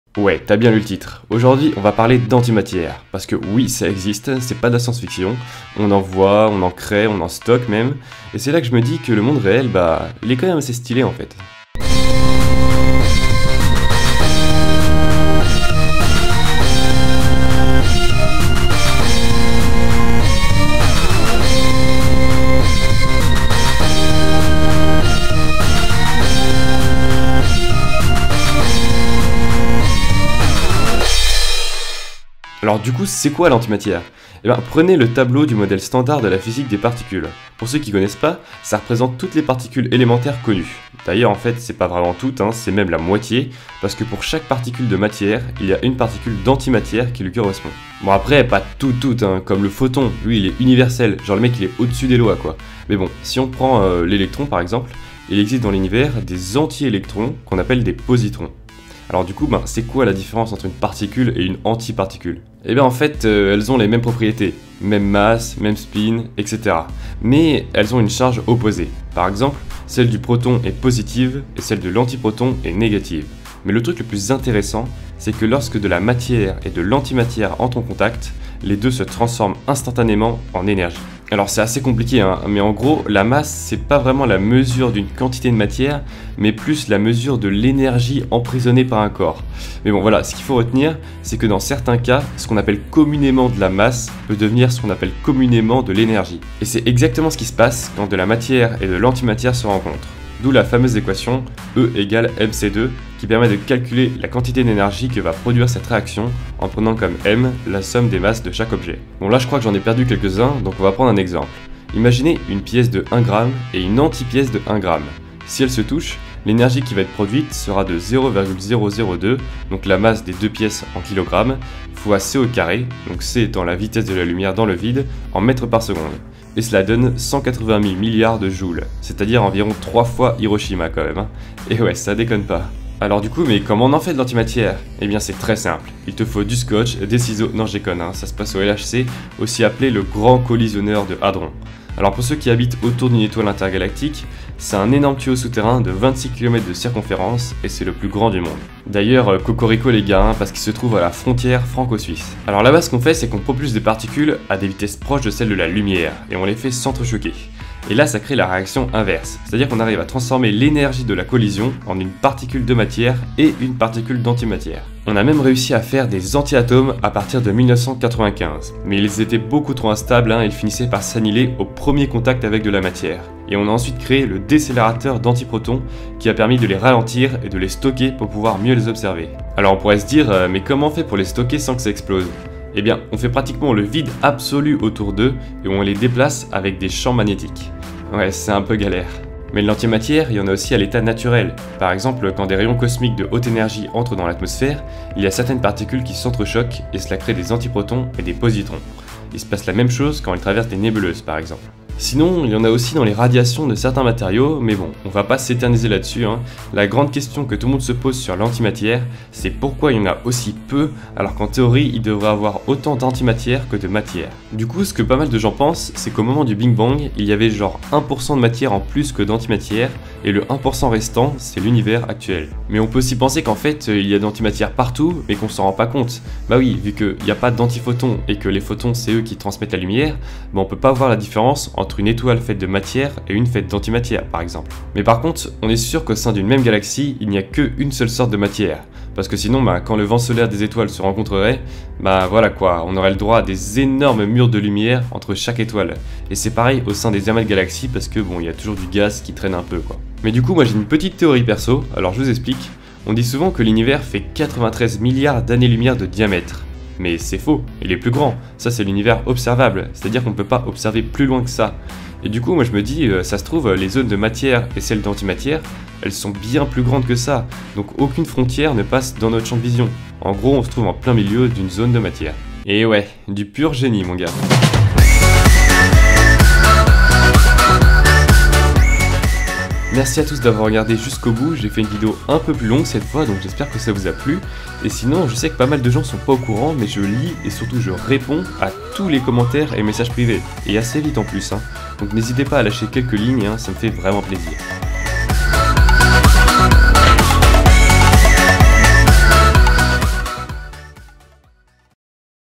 ► QU'EST-CE QUE L’ANTIMATIÈRE ? 💥❓
Aujourd'hui, je vous explique ce qu'est l'antimatière !

(Si tu vois ce message, c'est que tu es un petit chenapan !)